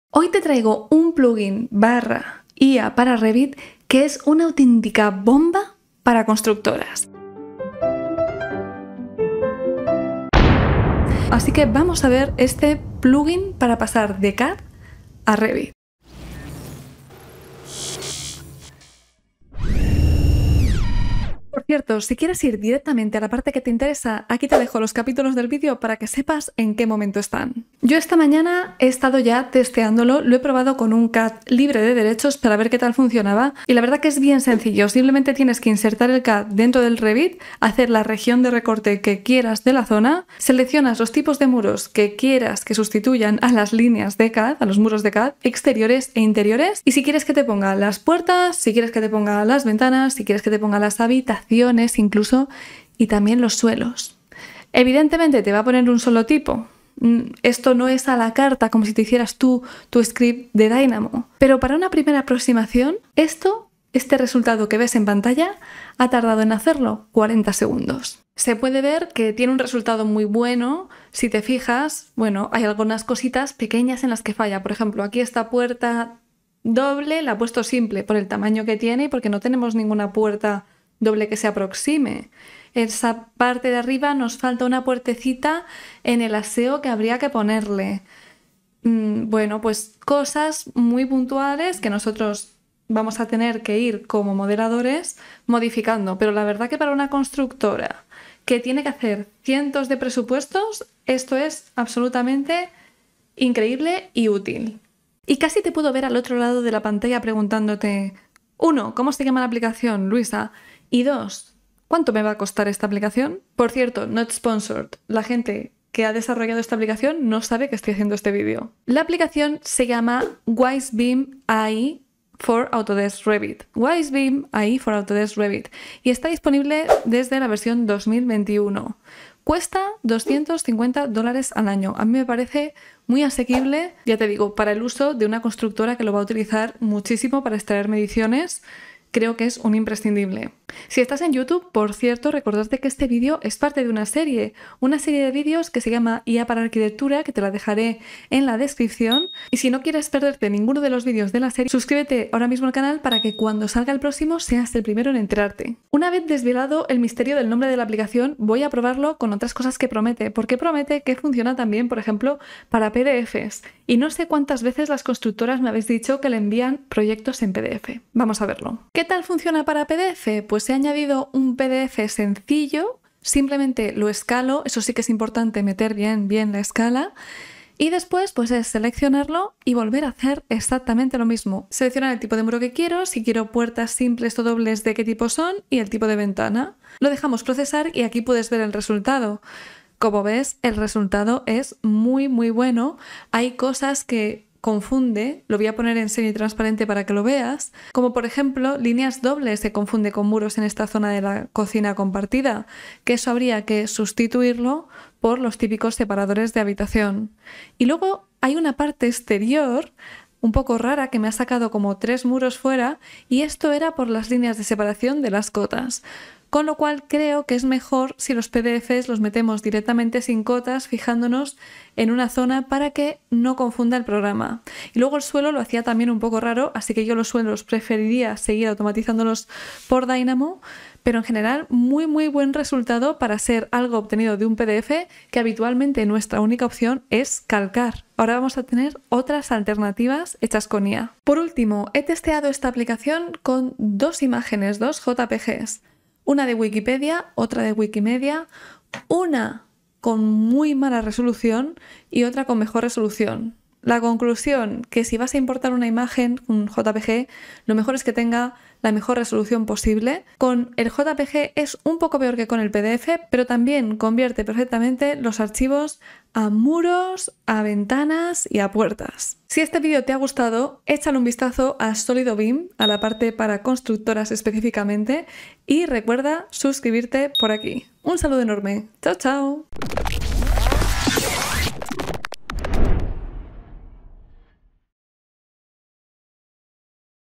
De CAD, pdf o jpg a REVIT en 1 click [ NUEVO PLUGIN con IA]
Si te has pasado horas traduciendo plantas de CAD a Revit esta inteligencia artificial va a convertirse en tu nueva mejor amiga. Siempre te traemos noticias frescas de la mano de Especialista3D.

📘 Nuestro libro "Salto al BIM"

💻 La tecnología que uso (ordenador, ratón, material de grabación)